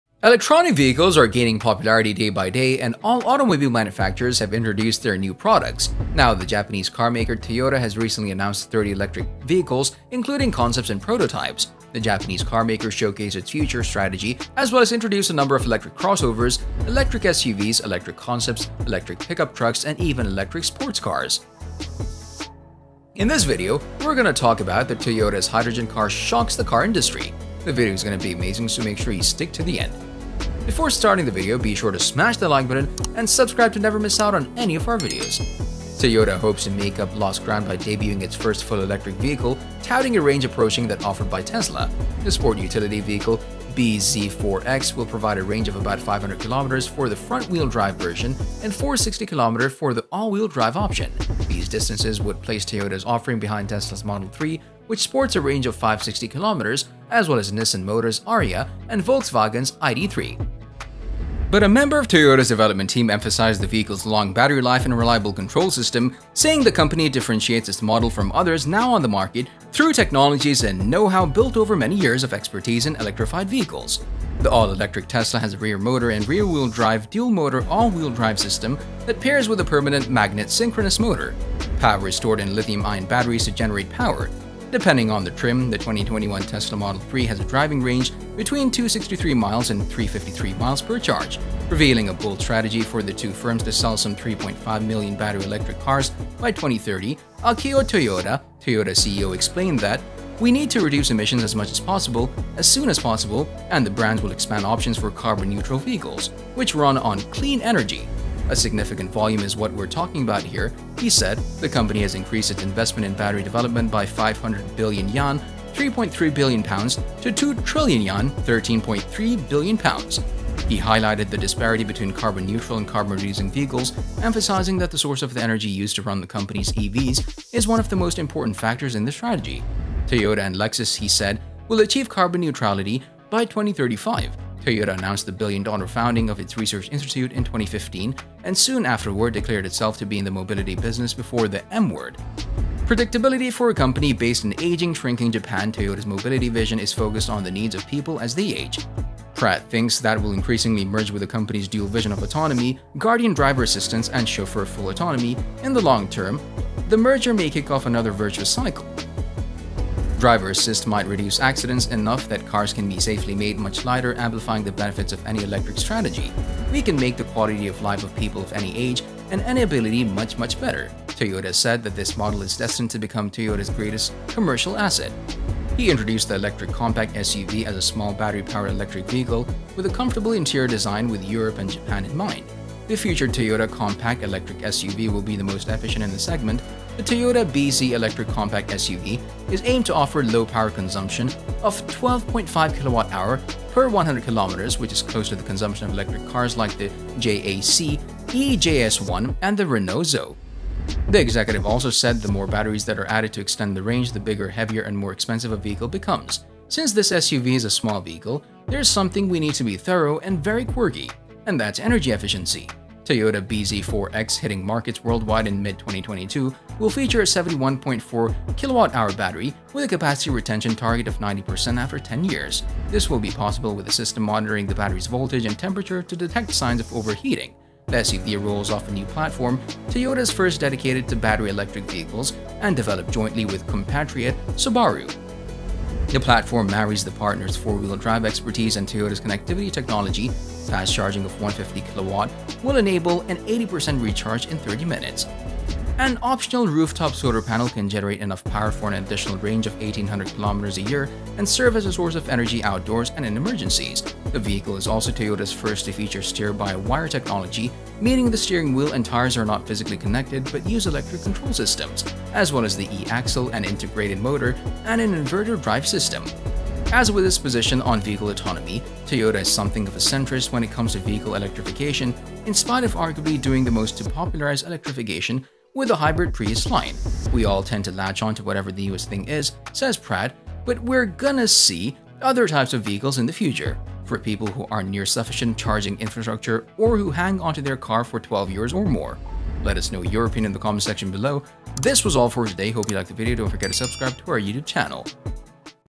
Toyotas Hydrogen Car SHOCKS The Car Industry
BMW and Toyota will team up to produce hydrogen fuel cell vehicles starting mid-decade, according to a report in Nikkei Asia.

The two will begin producing and selling hydrogen fuel cell vehicles developed jointly as early as 2025, BMW sales chief Pieter Nota told the publication.

The automakers have worked together before, jointly developing the iX5 Hydrogen based on BMW’s X5 SUV, as well as the BMW Z4 and Toyota Supra sports cars in 2019.

Toyota has deep experience with fuel cell technology, with its eight-year-old Mirai mid-size sedan heading into its second generation. Instead of an engine or a battery, these cars use hydrogen and oxygen to create electricity.

Fuel cells offer advantages over battery-electric vehicles. They can be refueled in three to four minutes and travel longer distances. However, the U.S. network of public hydrogen stations is concentrated in California, and even there it’s not yet ready for mass adoption.